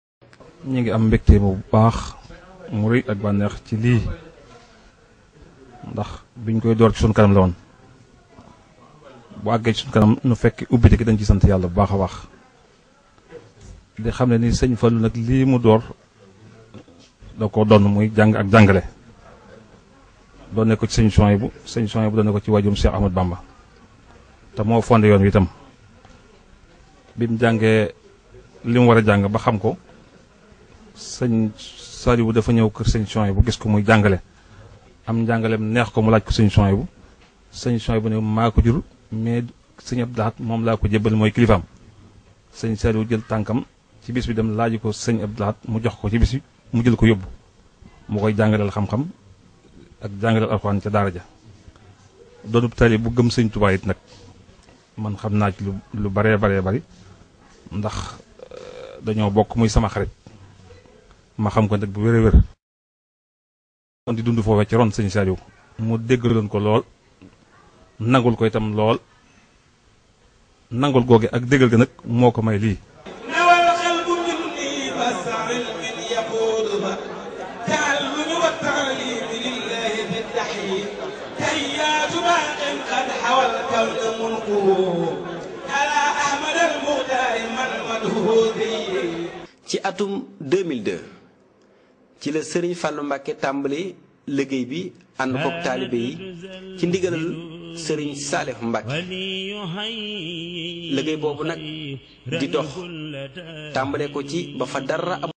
10-Archives: Ouverture Officielle de la Daara S. Chouhaybou MBACKE de S. Fallou Chouhaybou MBACKE
Archives: Ouverture Officielle de la Daara Serigne Chouhaybou MBACKE par Serigne Fallou Chouhaybou MBACKE à Grand Yoff 3 Dakar